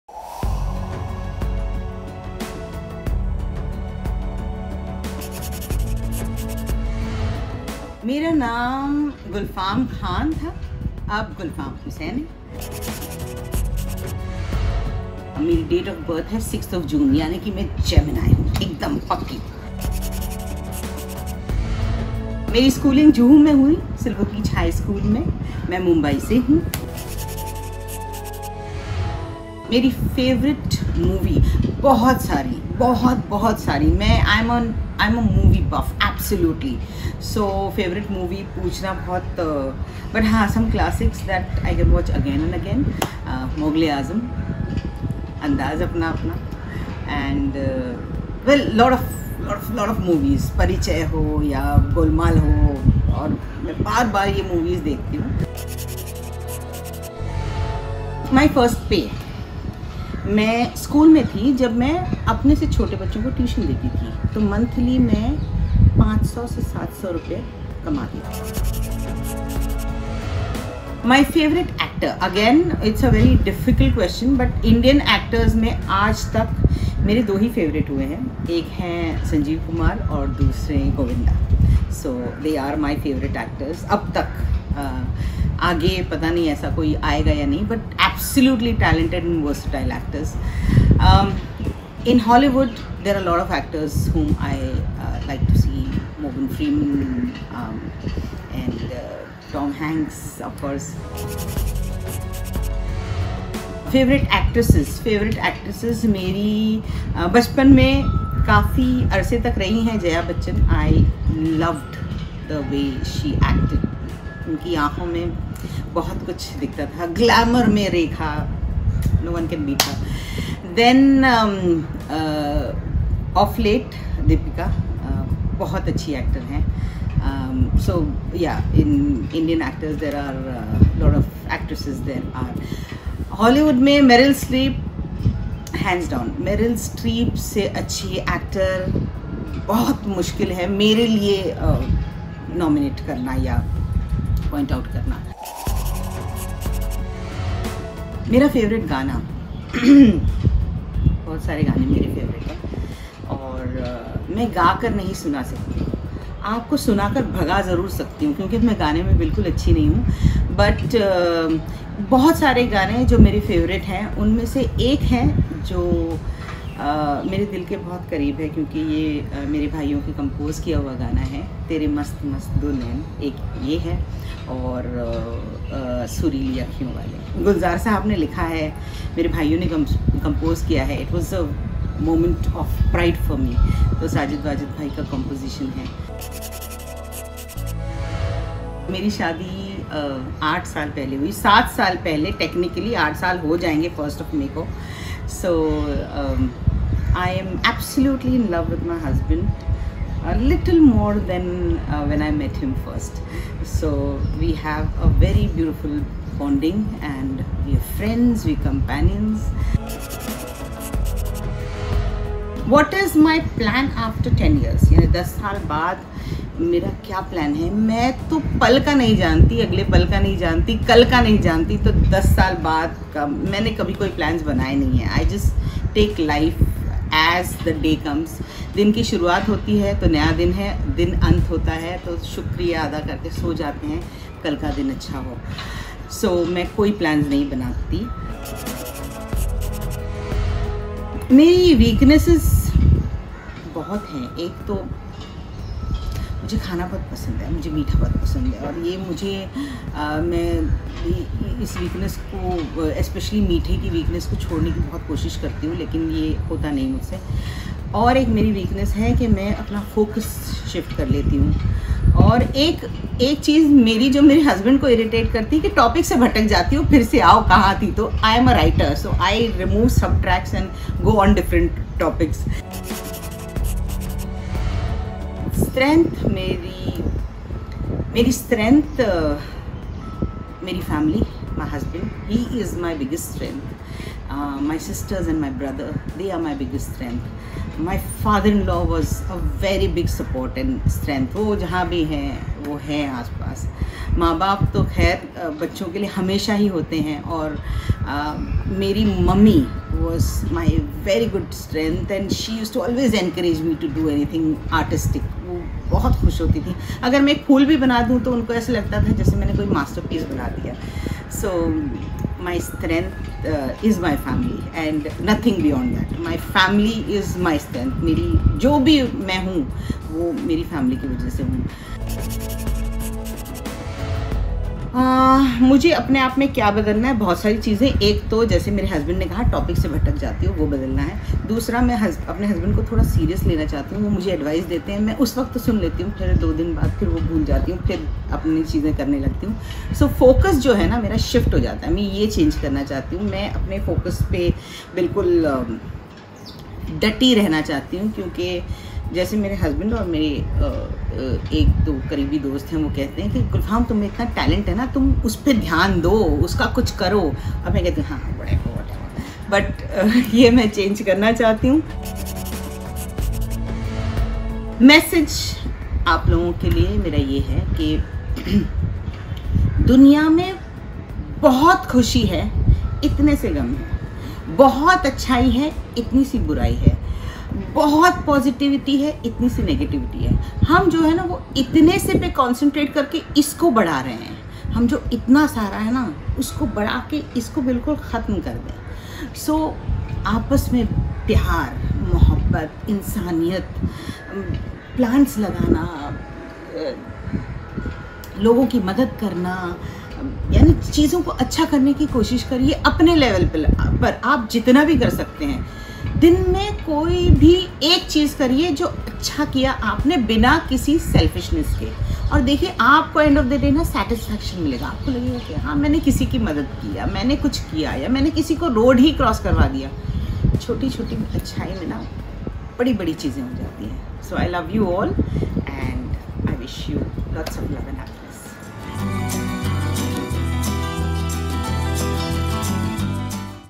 Gulfam Khan Hussain's Slam Book & Bio Data - Know your Stars !!
An unplugged Slam book of Gulfam Khan Hussain of Nikki Aur Jaadui Bubble where she is sharing the details of her life..